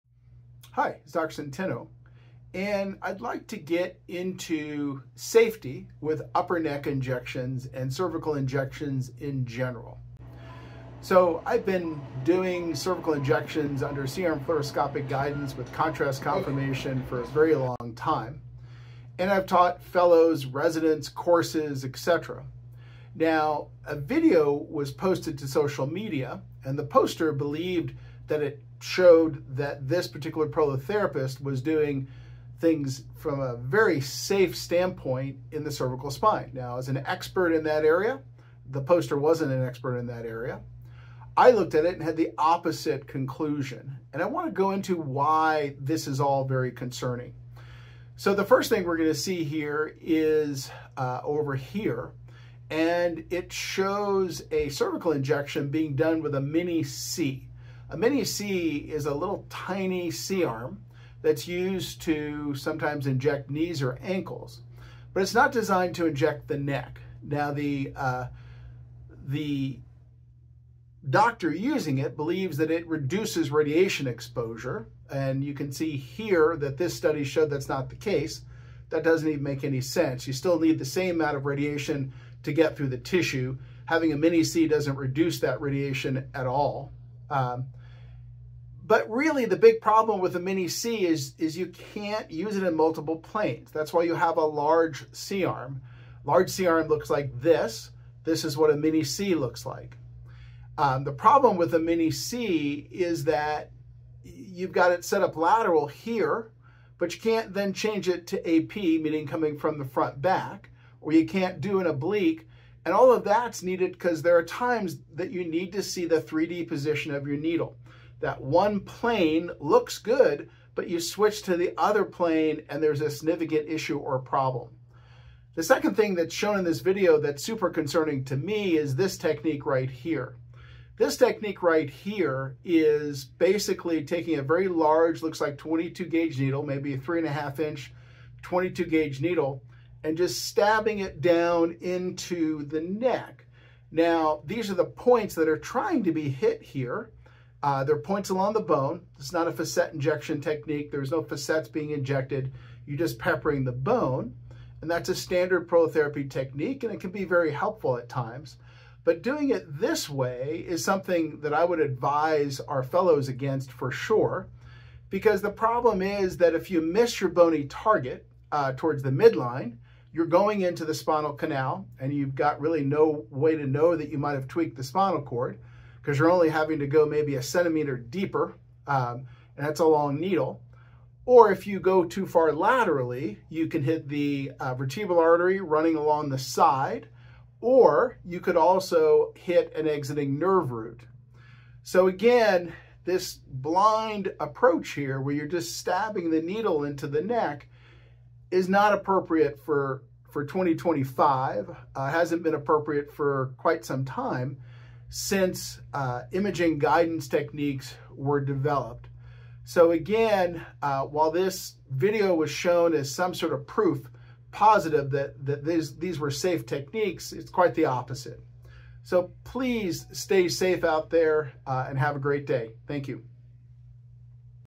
How Can You Stay Safe with Neck Prolotherapy Injections?
This video, presented by Dr. Centeno, explores the critical safety concerns associated with upper neck and cervical injections. Drawing from his extensive experience in fluoroscopically guided cervical injections, he analyzes a recently posted video that claims to demonstrate safe prolotherapy techniques. However, upon review, Dr. Centeno highlights several major issues that pose serious risks to patient safety.

One primary concern is the use of a mini C-arm for cervical injections. While some believe it reduces radiation exposure, research indicates otherwise. More importantly, the mini C-arm lacks the ability to capture multiple imaging planes, which is crucial for ensuring accurate needle placement. Without the ability to switch views, practitioners risk missing dangerous needle positioning that could lead to severe complications.

Dr. Centeno also critiques a blind injection technique shown in the video, where a long needle is rapidly inserted into the neck without real-time imaging guidance. This approach increases the risk of unintended damage to critical structures, such as the spinal cord, vertebral artery, or exiting nerve roots. Modern imaging techniques exist to prevent such risks, making this outdated method unacceptable in contemporary practice.

By providing this analysis, Dr. Centeno emphasizes the importance of using proper imaging guidance for cervical injections to ensure patient safety. He urges practitioners to adopt best practices and avoid techniques that introduce unnecessary risks.

Take your first step to recovering from CCI with our quiz

Centeno-Schultz Clinic
403 Summit Blvd Suite
201 Broomfield, CO 80021

Key Timestamps:
00:00 - Introduction to Safety in Cervical Injections
00:29 - Reviewing a Video Claiming Safe Injection Practices
00:56 - Concerns About Using a Mini C-Arm for Neck Injections
01:32 - Limitations of the Mini C-Arm in Multiple Planes
02:08 - Risky Needle Technique and Blind Injection Approach
02:50 - Potential Dangers of Missing the Bony Target
03:15 - Risks of Hitting Critical Structures Like the Vertebral Artery
03:37 - Final Thoughts on Safe Injection Practices